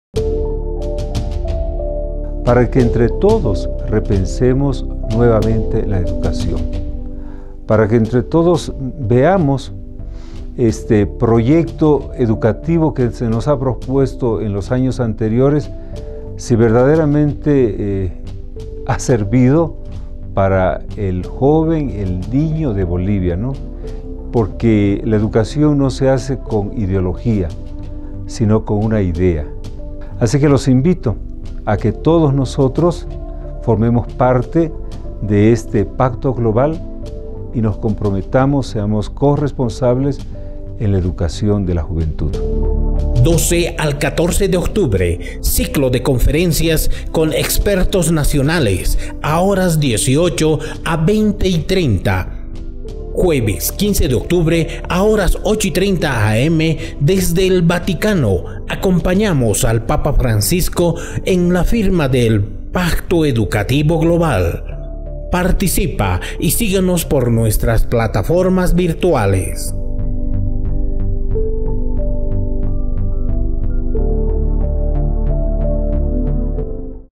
SPOT: PACTO GLOBAL EDUCATIVO
Invitación a las actividades relacionadas con la FIRMA DEL PACTO GLOBAL EDUCATIVO promovido por S.S. Francisco.
12, 13, 14 Y 15 de Octubre de 2020.
Producción: Diakonia Multimedia - CEB
Octubre 2020